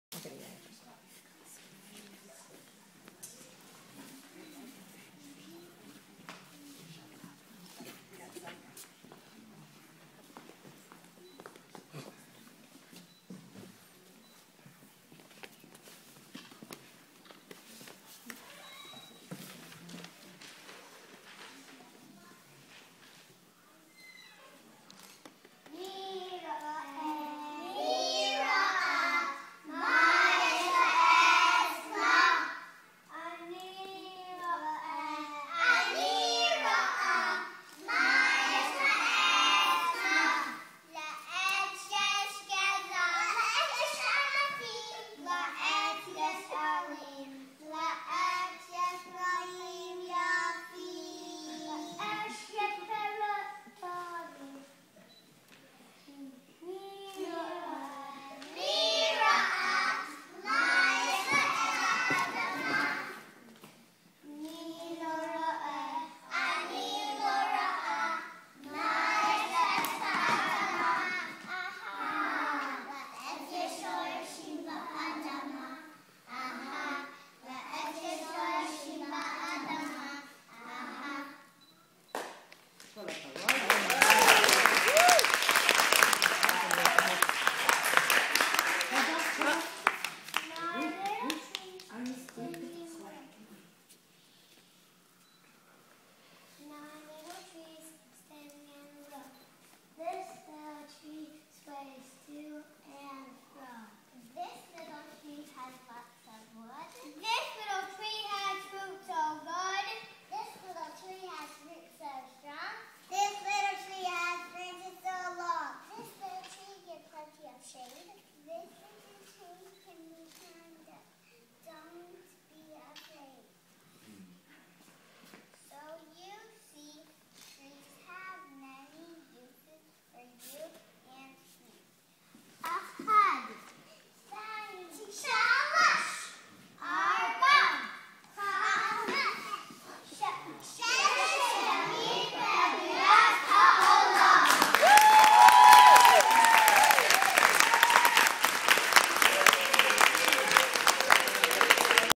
Kehila Kindergarten AVIGAL Presentation: Ode to Trees
Every year Kehila students honour Avigal, a woman who gave her time, home and compassion to nurture Israel young people. Our students study Israel and present their learning. This year the theme was trees.